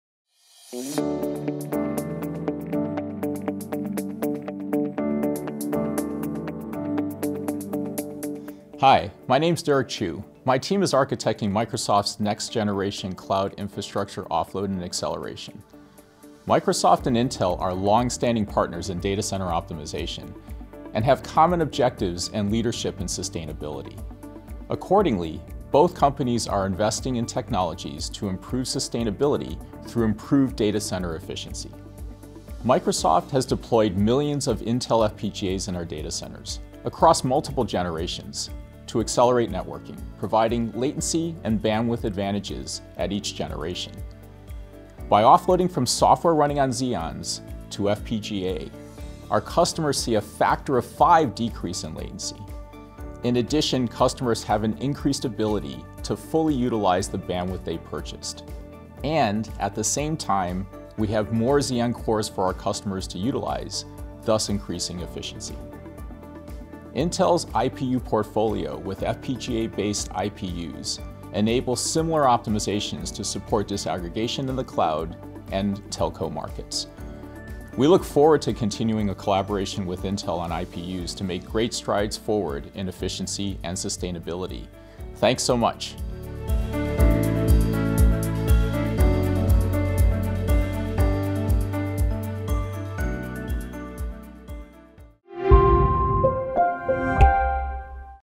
Optimizing Microsoft Data Center Networking with Intel® FPGAs
Learn how Microsoft deployed millions of Intel® FPGAs to improve efficiency and decrease latency to support disaggregation in cloud and telco markets.